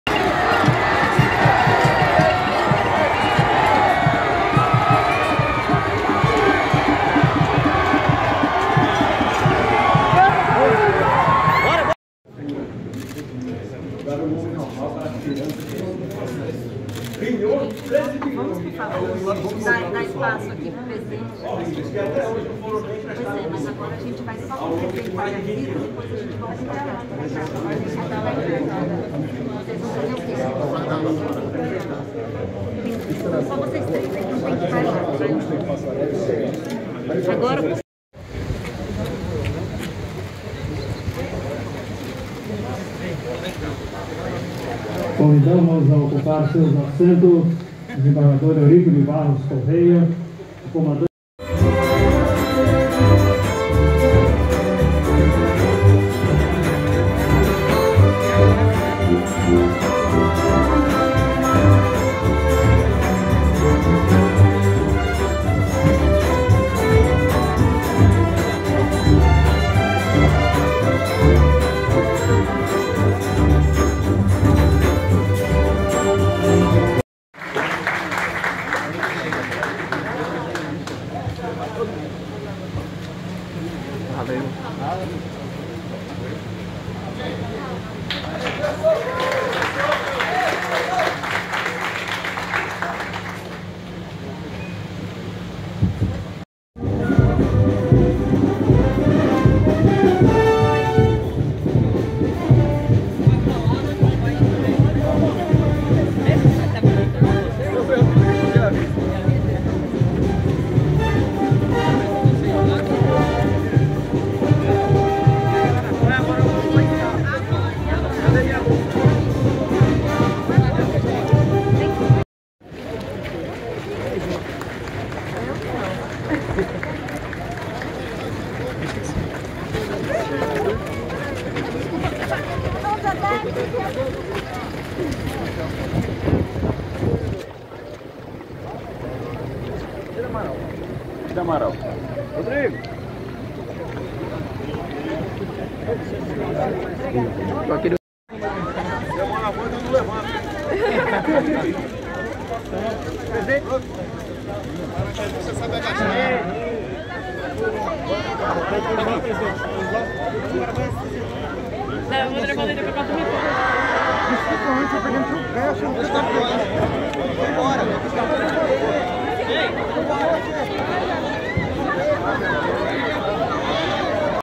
PRESIDENTE BOLSONARO CHEGA AO RECIFE E É RECEBIDO POR APOIADORES
PERNAMBUCO NA TV